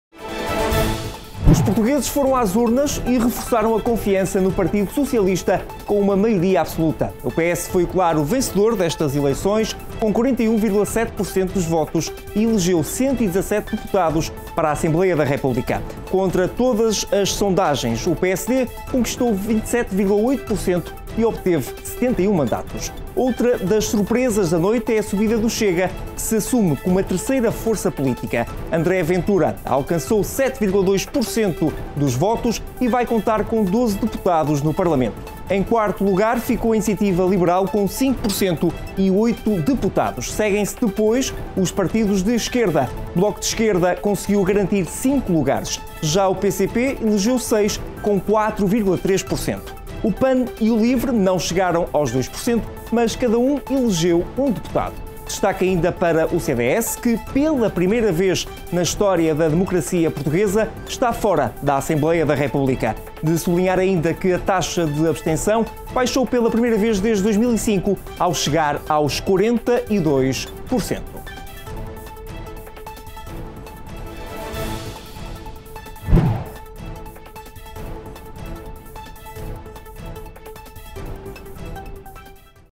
Eleições Legislativas 2022
Os portugueses foram às urnas e reforçaram a confiança no Partido Socialista com uma maioria absoluta. O PS foi o claro vencedor destas eleições com 41,7% dos votos e elegeu 117 deputados para a Assembleia da República. Contra todas as sondagens, o PSD conquistou 27,8% e obteve 71 mandatos.